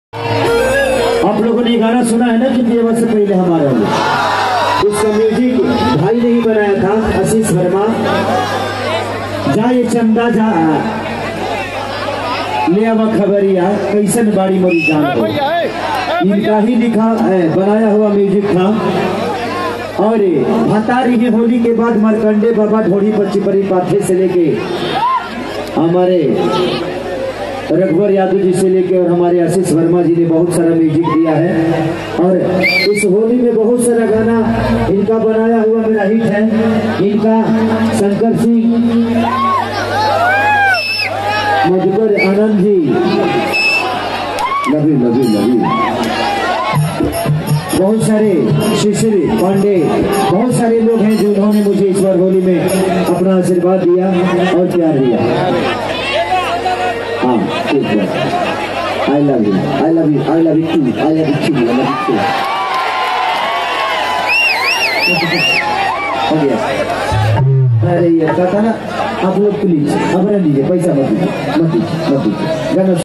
Khesari lal yadav ne music director ashish verma aur raghubar yadav ko kya kaha
Sonu pandey